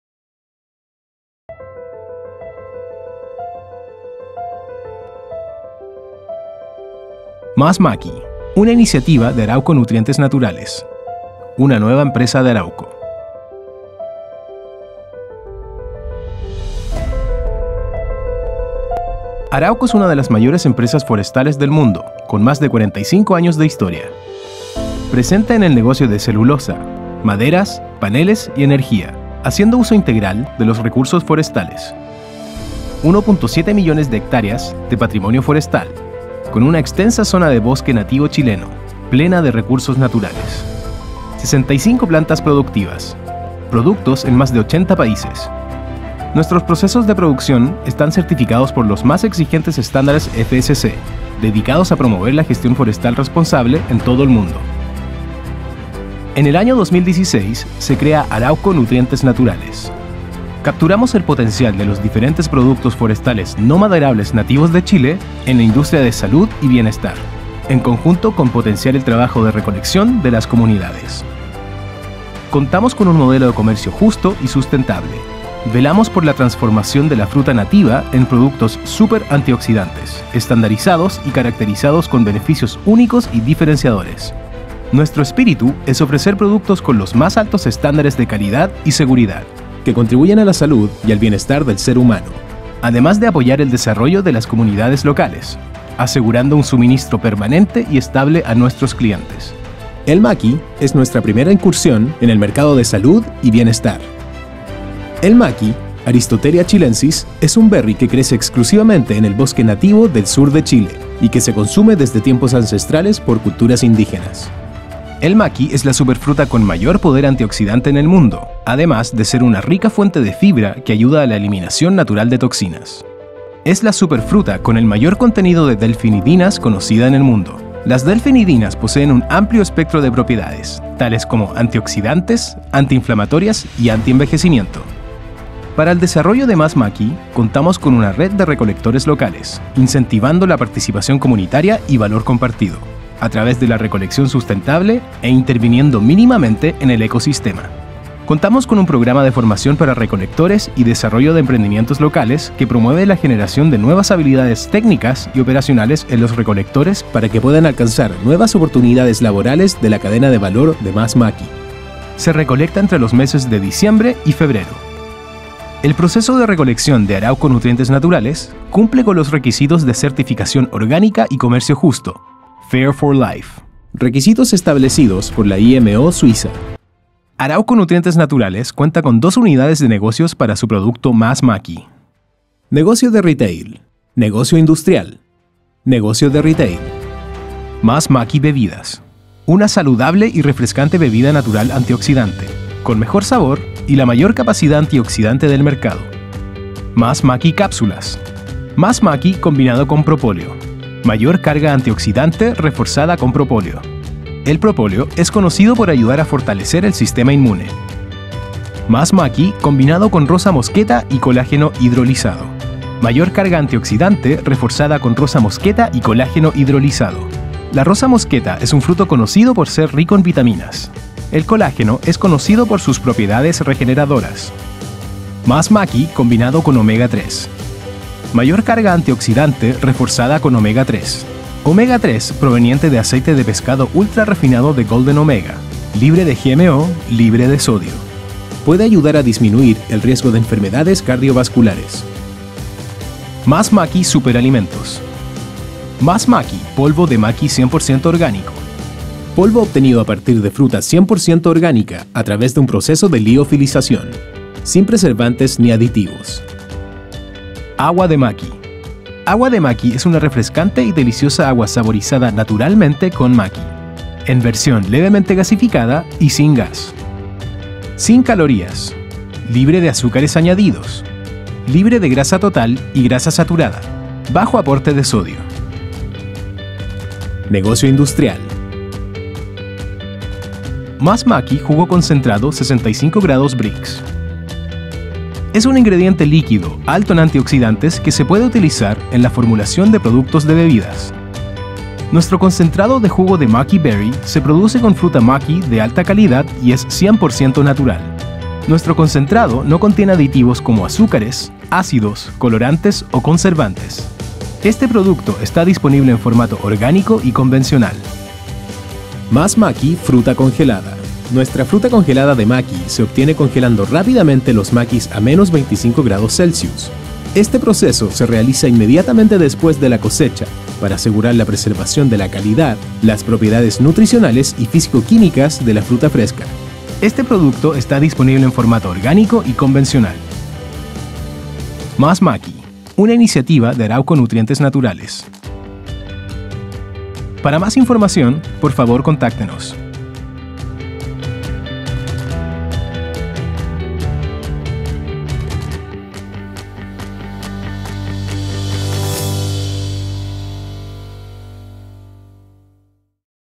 MASMAQUI MASTER ESP v2
Video explicativo del producto de innovación de Arauco MasMaqui